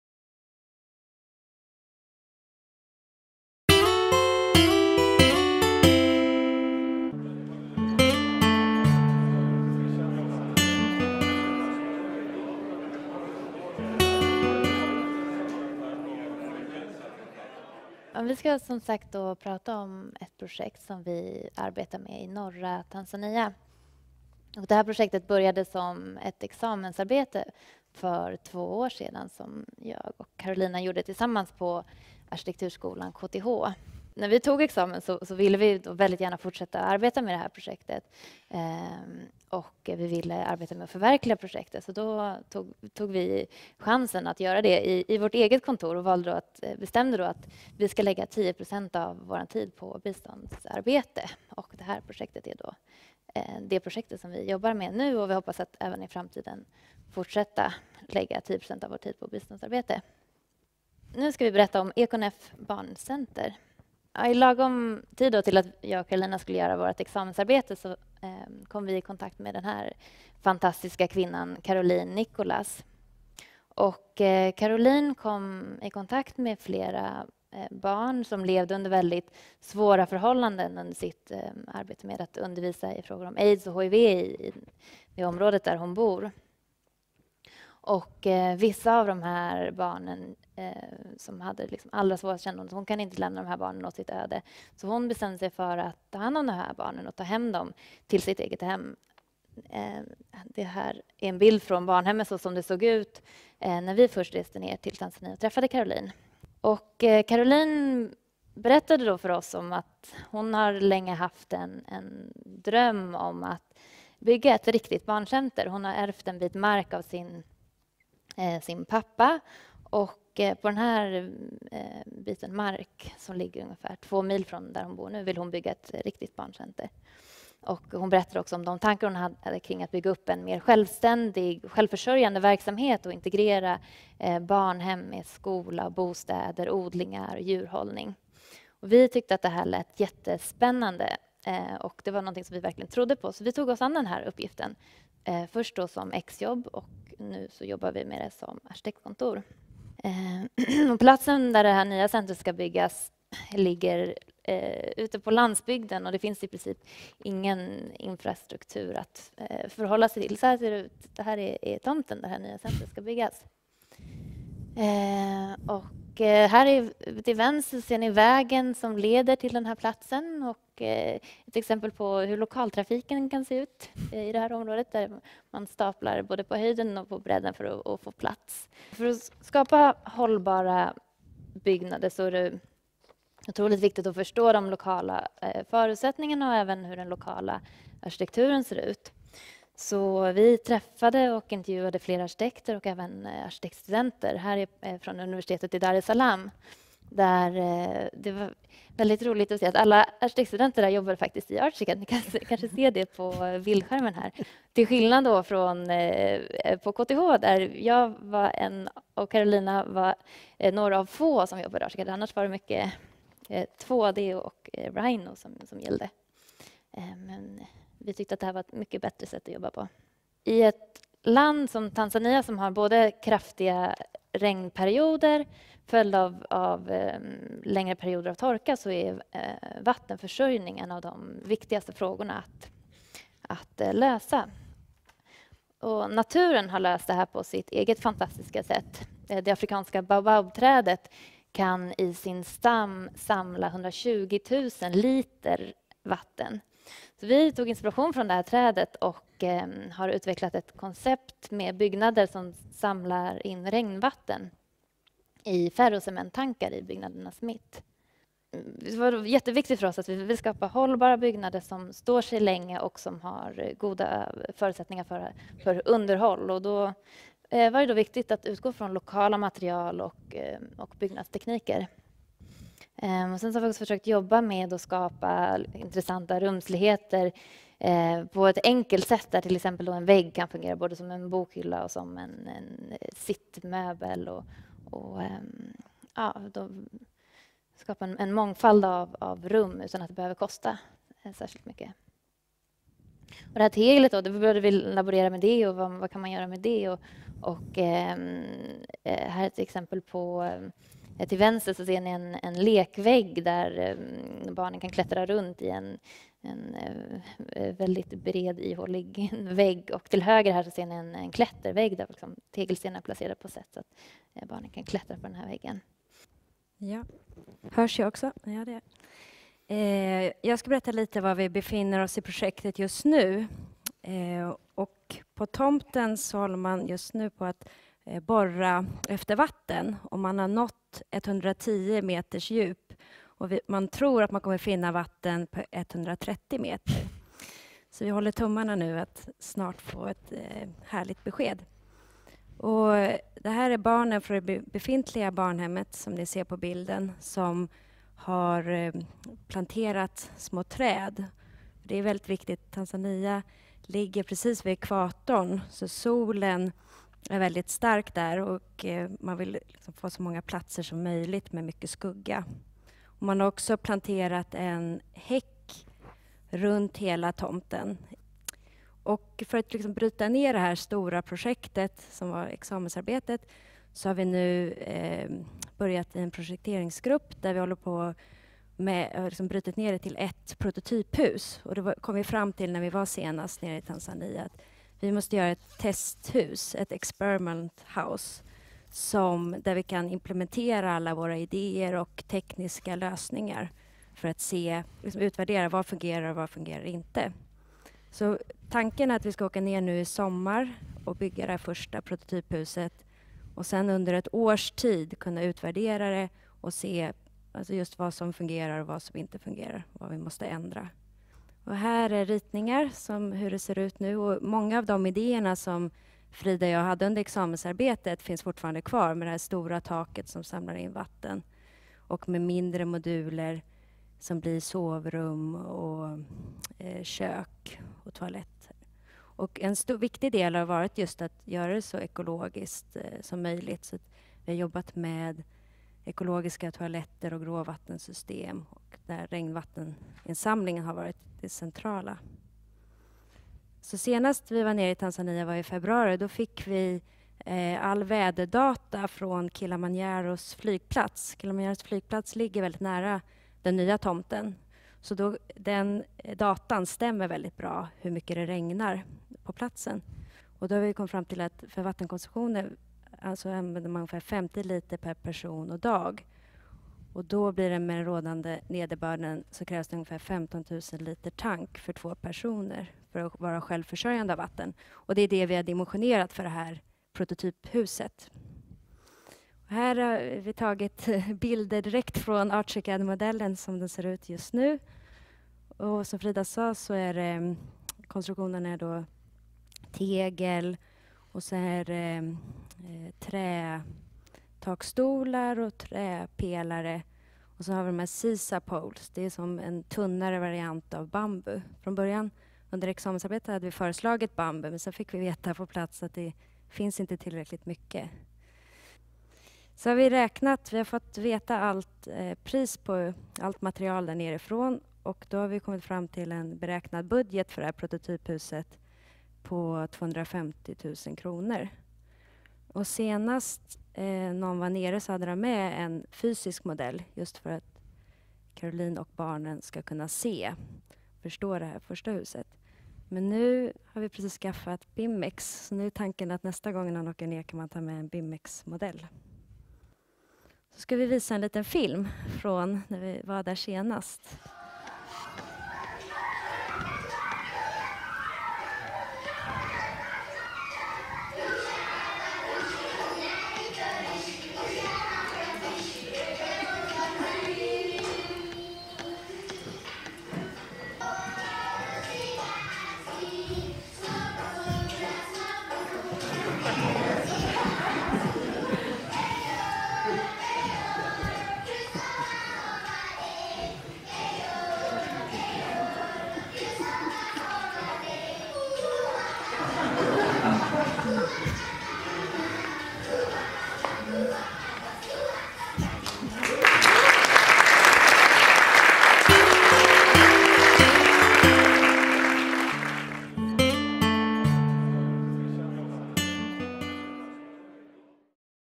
Asante Arkitektur & Design - Arkitekter utan gränser och Ingenjörer i Tanzania
Carolina Wikström och Frida Öster från Asante Arkitektur & Design berättar om sitt projekt. Ett examensarbete som blivit ett omtalat projekt i Tanzania. I samarbete med Arkitekter utan gränser och Ingenjörer utan gränser har man utvecklat en lösning som i princip är självförsörjande. Läs mer om detta på